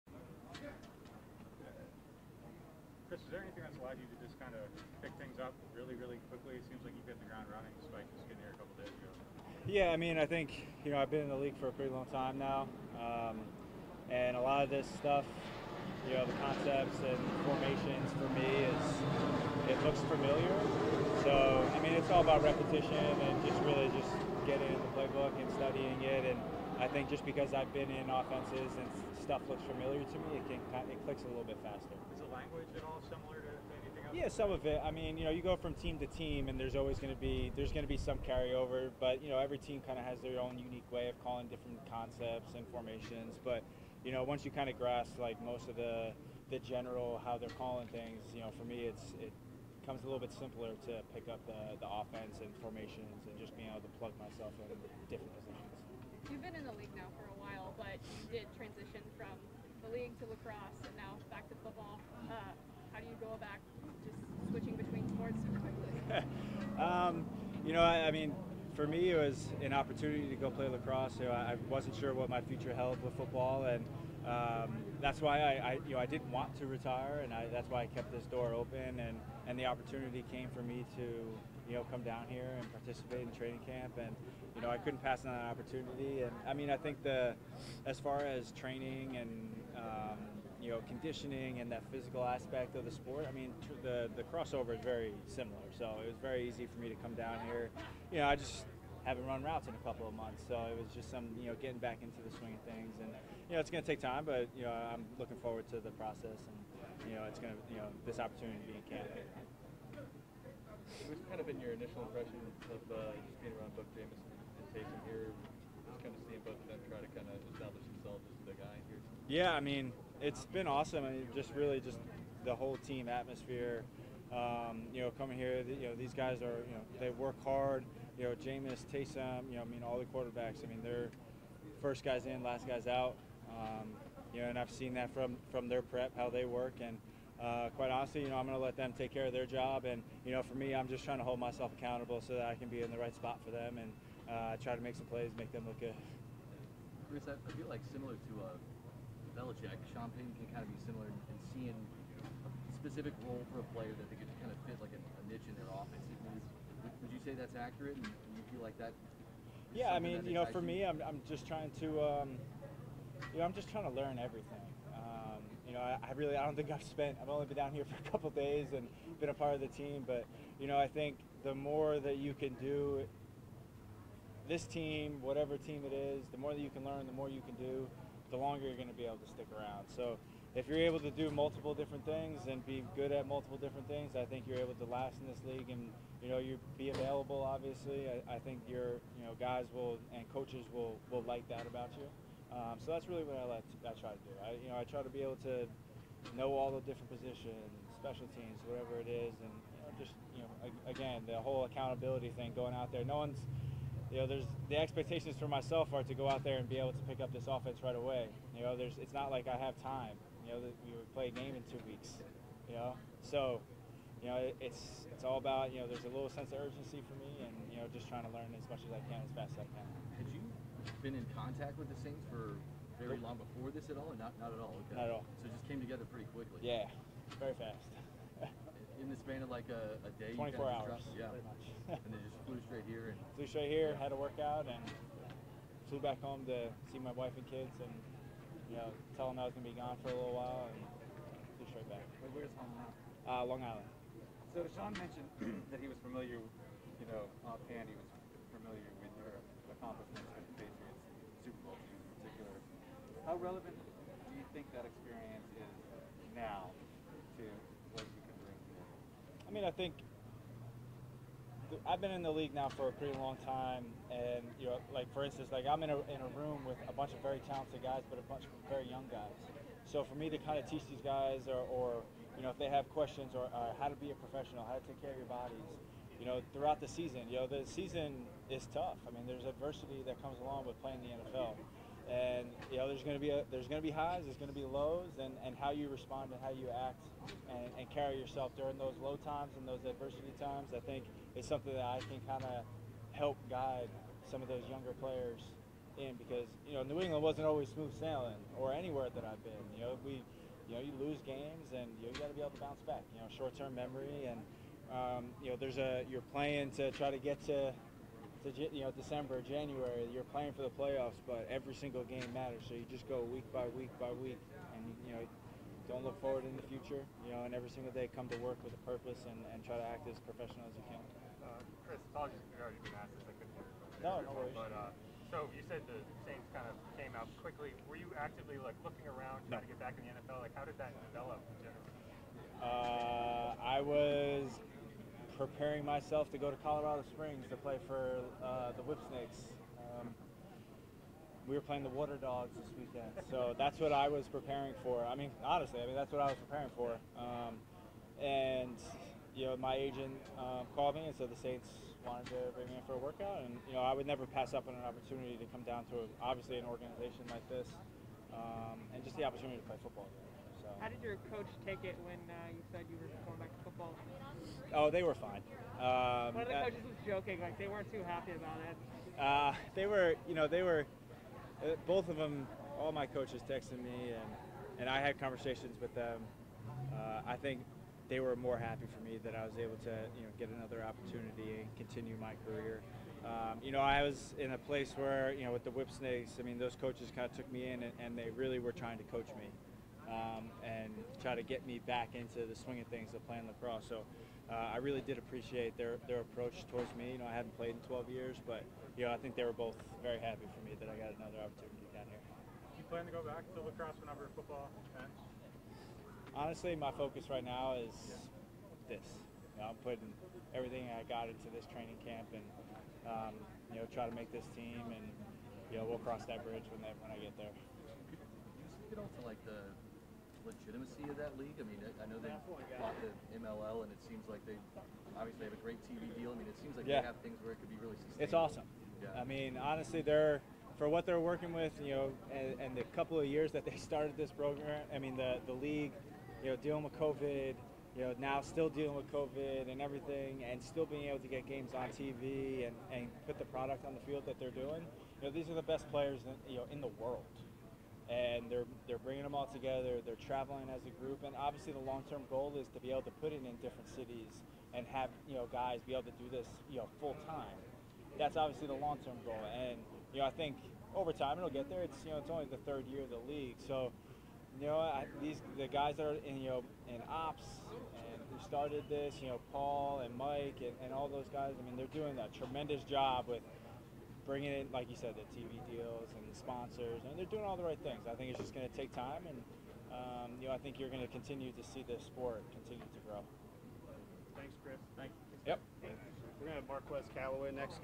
Chris Hogan talks lacrosse, offseason conditioning | Saints Training Camp 2021
New Orleans Saints wide receiver Chris Hogan interview from Day 2 of Saints Training Camp 2021 on July 30, 2021.